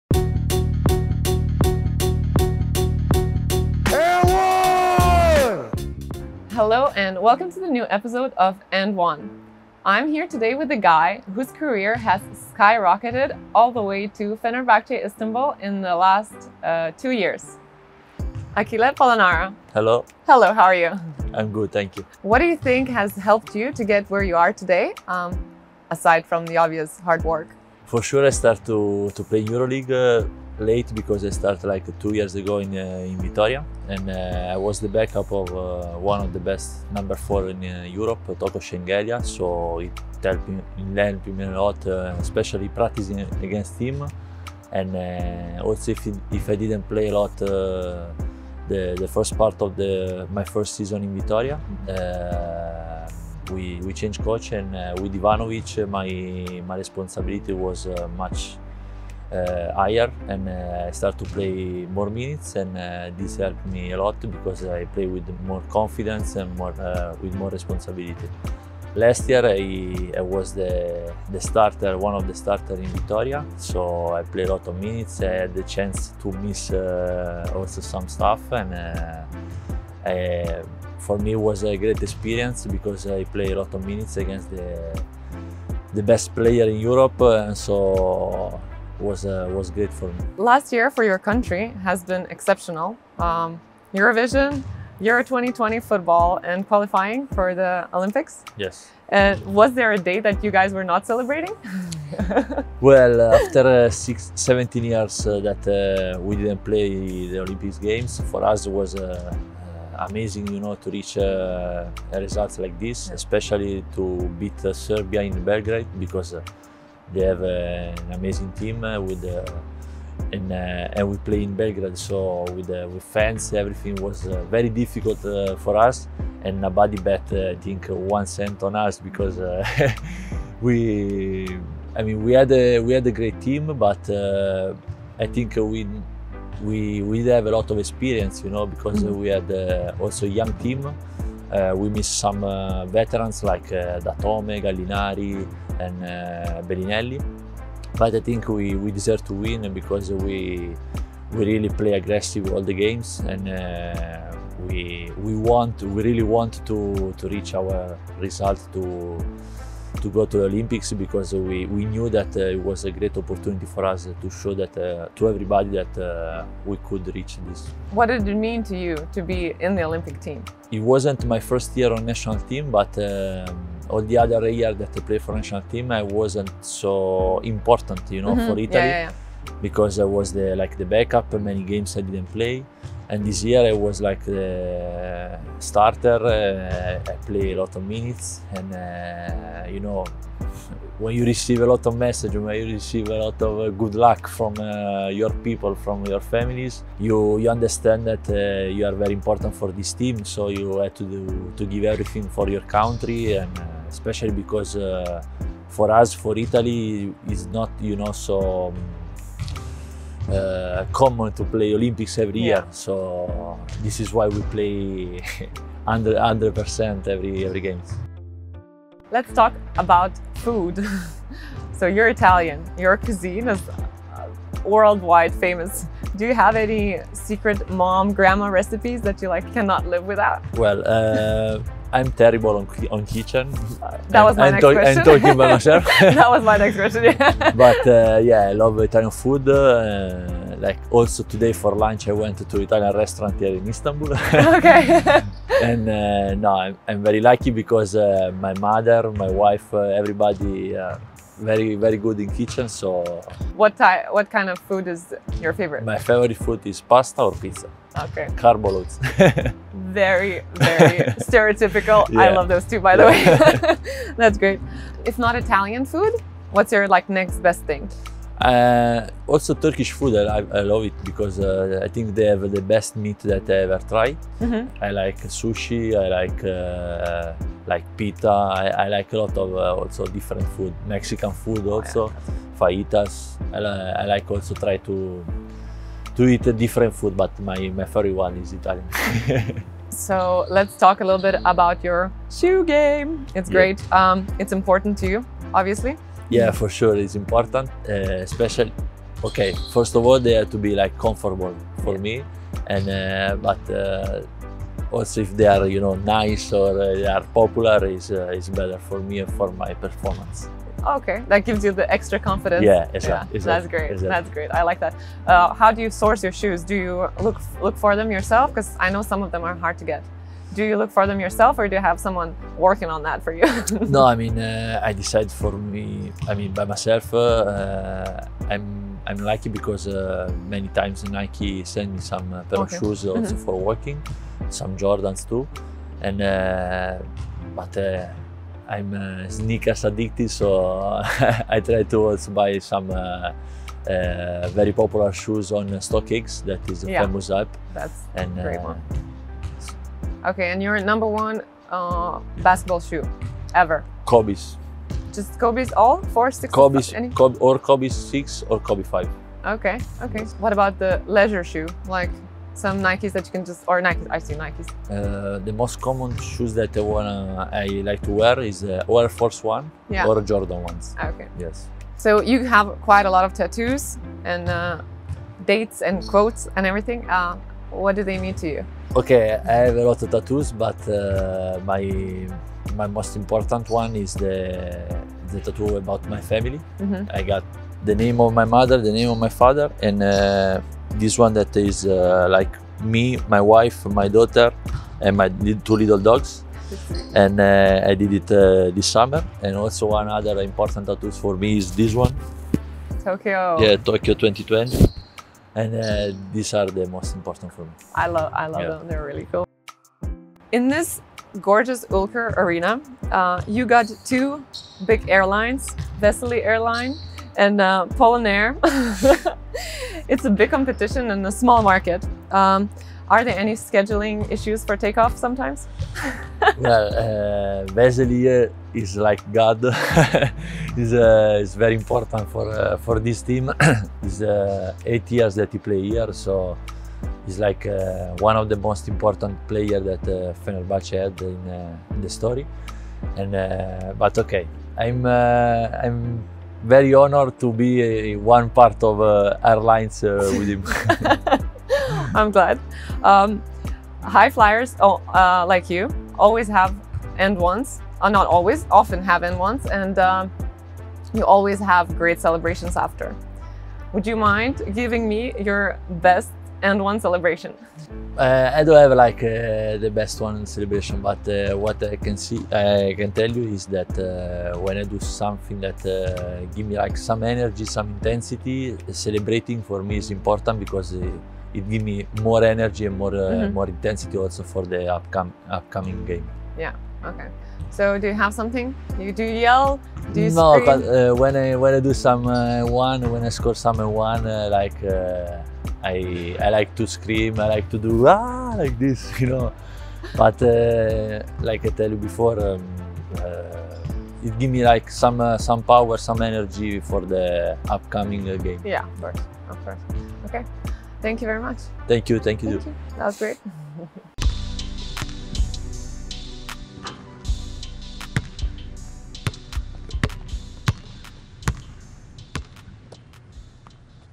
“I’m a Sneakers Addict” | And One with Achille Polonara
Martyna Motum presents another edition of And One! This time she talks to Fenerbahce Istanbul's forward Achille Polonara about his rise to EuroLeague with Saski Baskonia, his experience at the Tokyo 2020 Olympics with the Italian national basketball team, and the importance of sneakers in his life.

- Intro & his rise with Baskonia (0:00);
- Representing Italy at the Olympics (1:30);
- Unsurprising favorite cuisine (3:39);
- Sneakers addiction (4:50);
- Achille's favorite tattoos (6:12);
- PolonAir & Vesely Airlines (6:50);
- How Achille celebrates an "And One" (7:33).